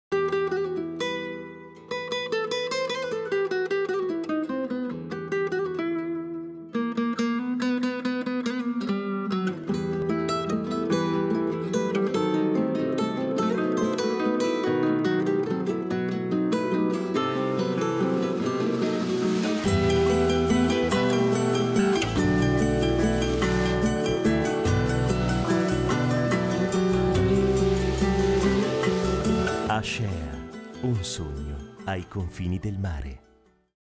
SPOT Ascea Elea Velia Music by Sursumcorda
Musica originale nei vostri SPOT:
CONTATTATE info [et] sursumcorda.it
Spot commissionato dal Comune di Ascea. Musica Sursumcorda.
Regia Isabella Giossi